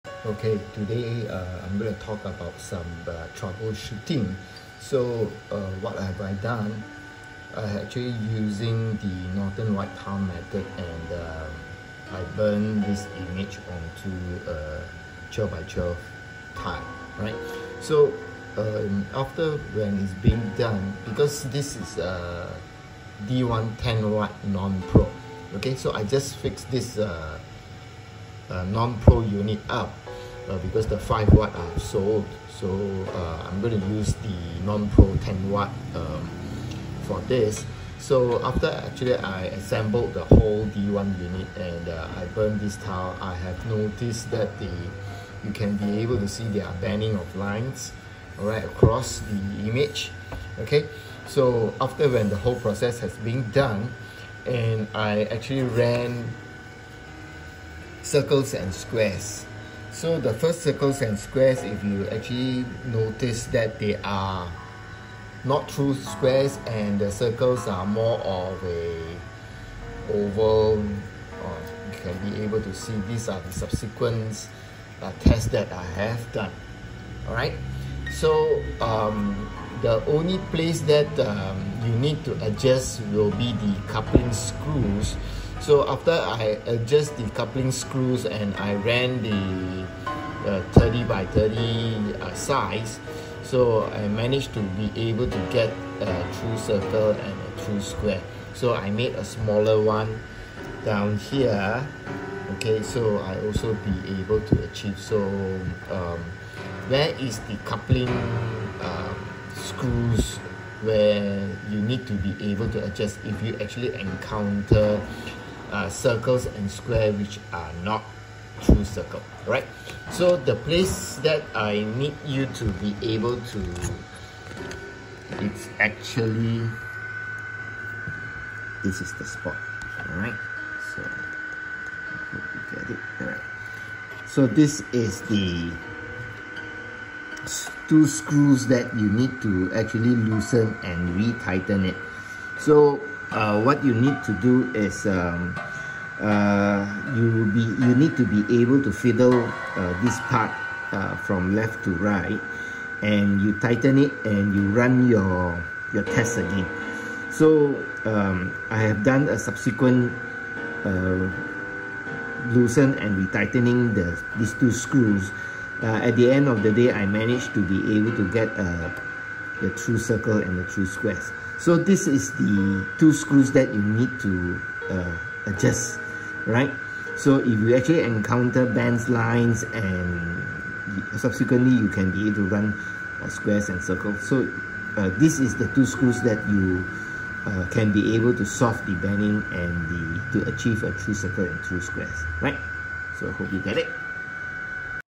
How to solve banning lines issue when engraving on Xtool D1
Step by step how to solve the issue on lines when engraving on Xtool D1 or D1 Pro.